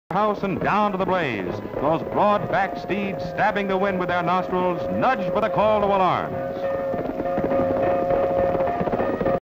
1920s Horse Drawn Firetruck
Three horse-drawn fire engines leave the station

firefighters, fire engines, steam power, horses, wagons, pumper trucks, firemen